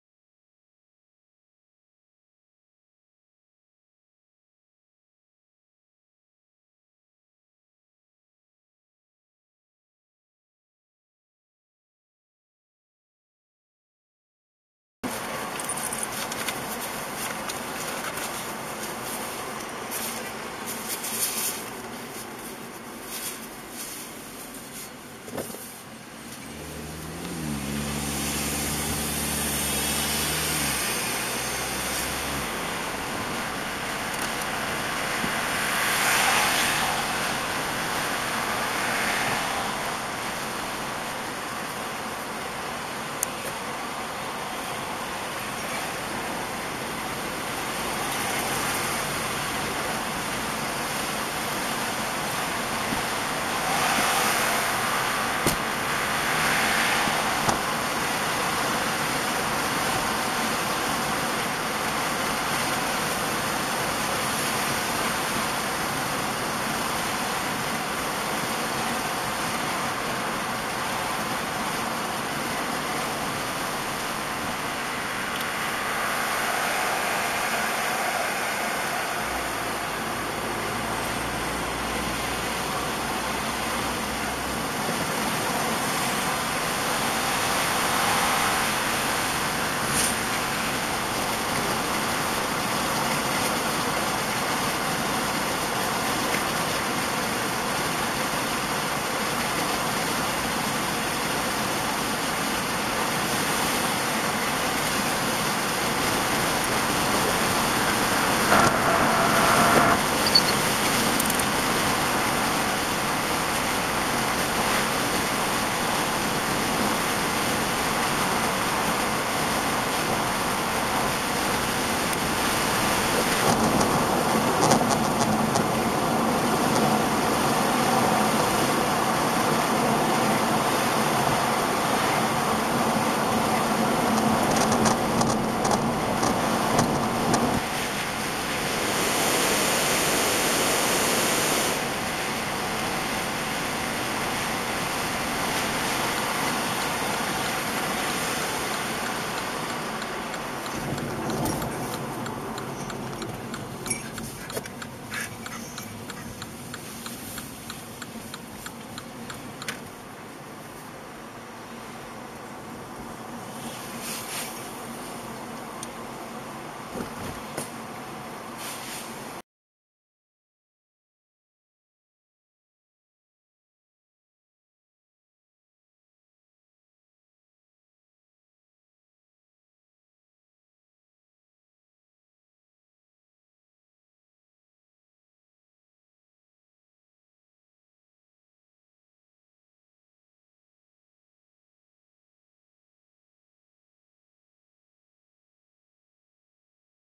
All Green All the Way, communicate with traffic lights
Respect, follow and communicate with the traffic light with your calm, humble and peaceful mind.  It will bring you closer to your destination. :D